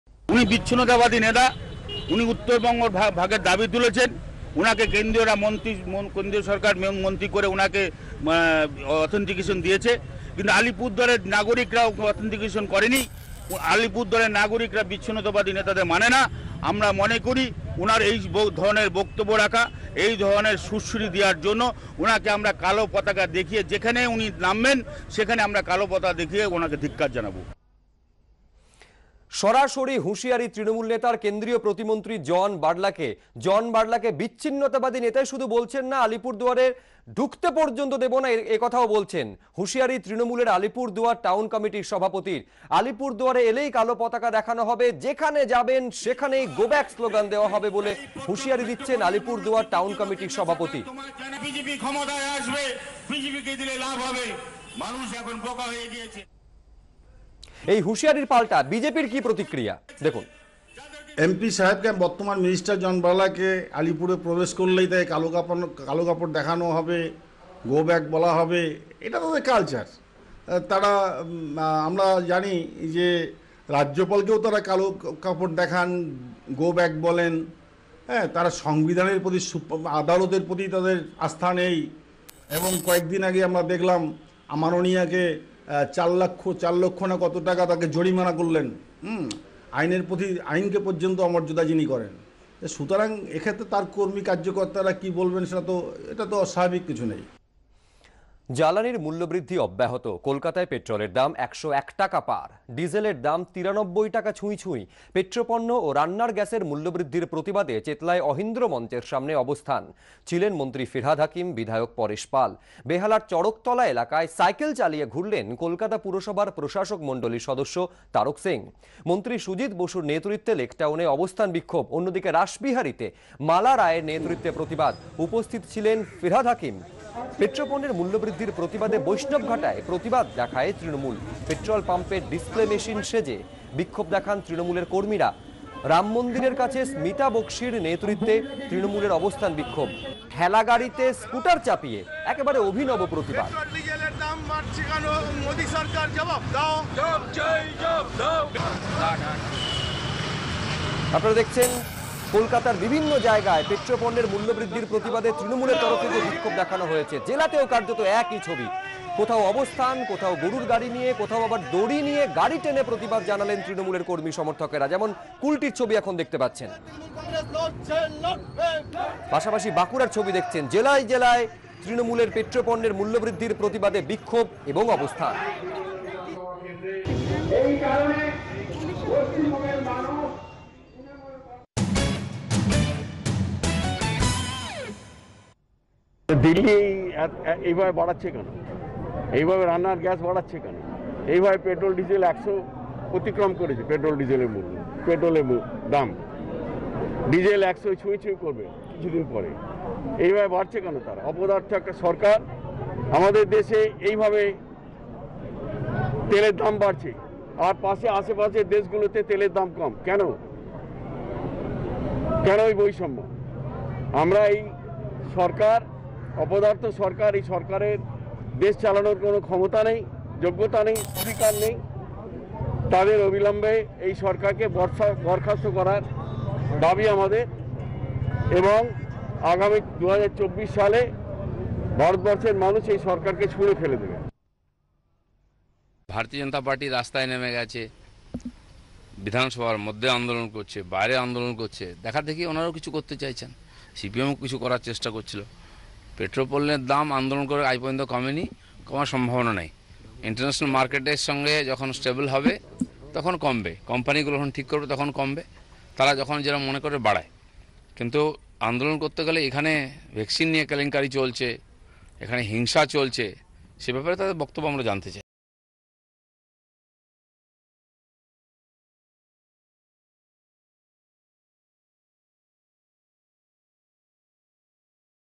John Barla: 'আলিপুরদুয়ারে এলেই জন বার্লাকে কালো পতাকা দেখানো হবে', হুঁশিয়ারি তৃণমূলের
কেন্দ্রীয় প্রতিমন্ত্রী জন বার্লাকে হুঁশিয়ারি তৃণমূল নেতার।‘জন বার্লা বিচ্ছিন্নতাবাদী নেতা, আলিপুরদুয়ারে ঢুকতে দেব না’, হুঁশিয়ারি তৃণমূলের আলিপুরদুয়ার টাউন কমিটির সভাপতির। তিনি বলেন, ‘আলিপুরদুয়ারে এলেই কালো পতাকা দেখানো হবে। যেখানেই যাবেন, গোব্যাক স্লোগান দেওয়া হবে। জন বার্লা একজন বিচ্ছিন্নতাবাদী নেতা। তিনি উত্তরবঙ্গ ভাগ করা দাবি তুলেছেন। কেন্দ্রীয় সরকার তাঁকে মন্ত্রী বানিয়ে ক্ষমতা দিয়েছে। কিন্তু আলিপুরদুয়ারের নাগরিকরা তাঁকে এই ক্ষমতা দেয়নি, দেবেও না। আলিপুরদুয়ারের নাগরিকরা বিচ্ছিন্নতাবাদকে সমর্থন করে না।’ এই প্রসঙ্গে বিজেপির তরফে প্রতিক্রিয়া, ‘আমরা জানি তৃণমূল রাজ্যপালকেও কালো পতাকা দেখায়, গো ব্যাক বলে। তৃণমূলের আইন আর সংবিধানের প্রতি কোন আস্থা নেই। তাঁদের কাছ থেকে আর কী আশা করা যায়।’